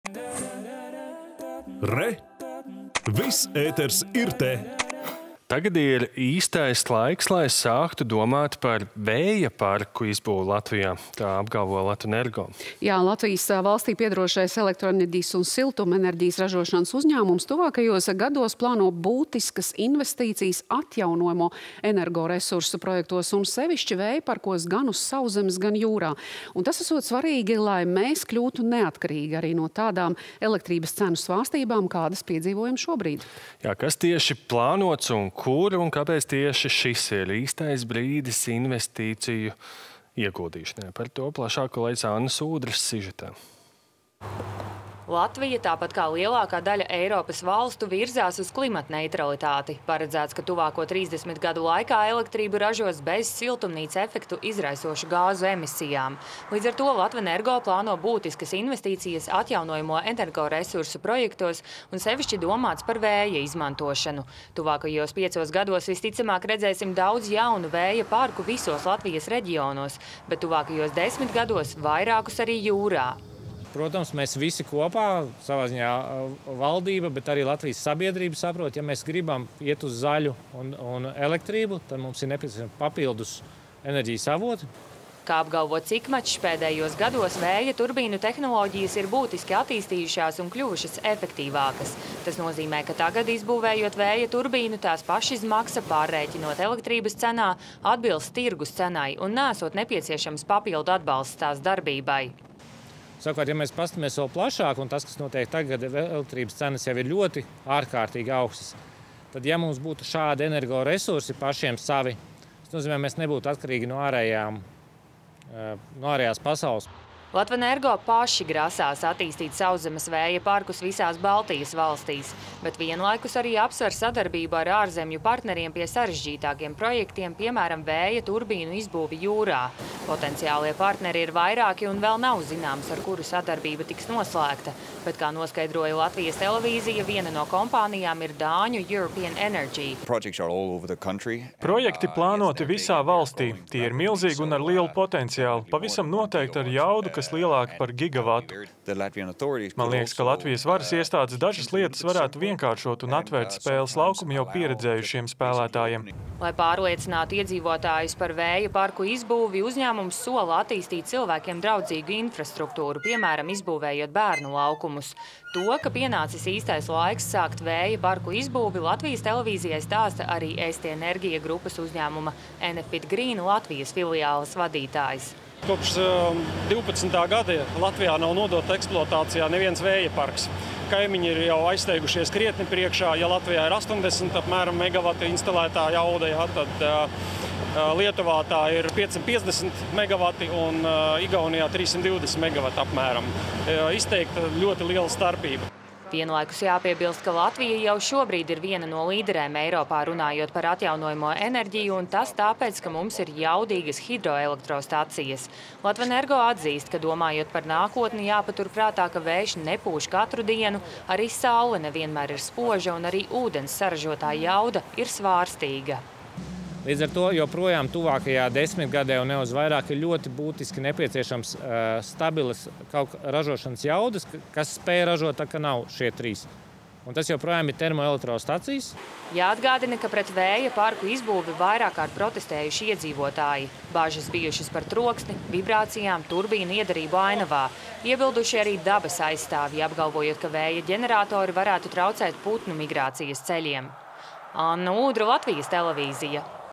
“Latvenergo”: Pienācis īstais laiks vēja parku izbūvei
Tagad ir īstais laiks, lai sāktu domāt par vēja parku izbūvi Latvijā – tā apgalvo “Latvenergo”.
Latvijas valstij piederošais elektroenerģijas un siltumenerģijas ražošanas  uzņēmums tuvākajos gados plāno būtiskas investīcijas atjaunojamo energoresursu projektos un sevišķi vēja parkos gan uz sauszemes, gan jūrā. Tas esot svarīgi, lai mēs kļūtu neatkarīgāki arī no tādām elektrības cenu svārstībām, kādas piedzīvojam tagad.
Kas tieši plānots un kur, un kāpēc šis ir īstais brīdi investīcijām, par to plašāk Annas Ūdres sižetā.